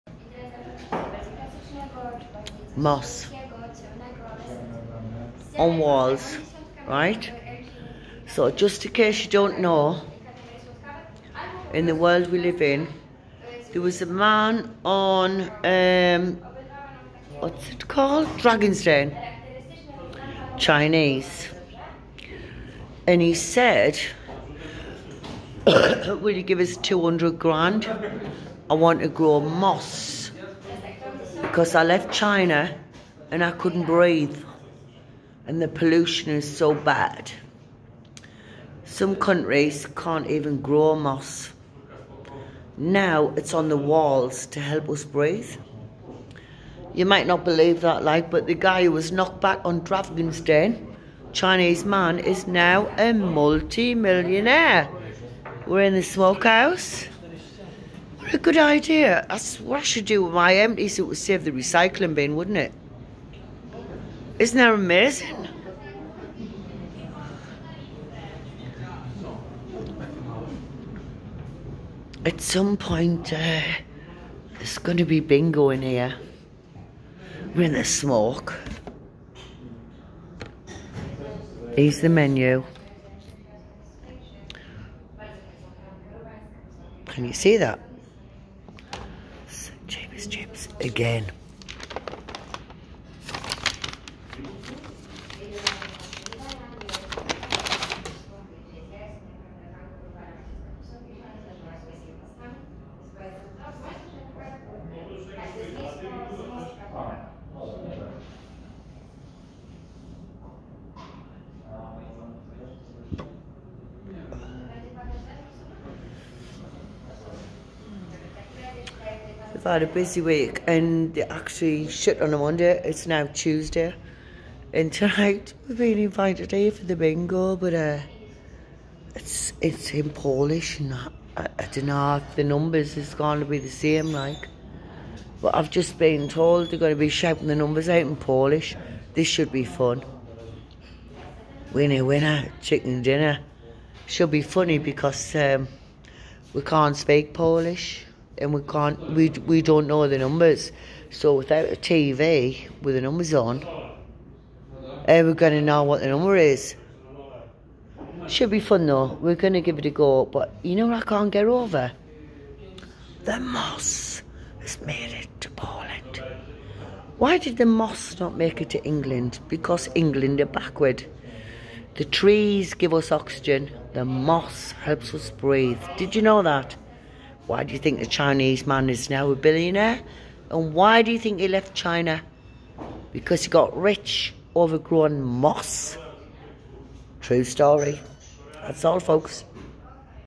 Dragon's Den Moss. Smoke Bar best in WROCLAW. Food and Real ale.
15 real ales and quality food at SMOKE IN WROCLAW.  FIND them on Facebook SMOKE-PASTRAMI or insta smokepastrami.. highly recommended.. MOSS!! Huge TV downstairs wonderful staff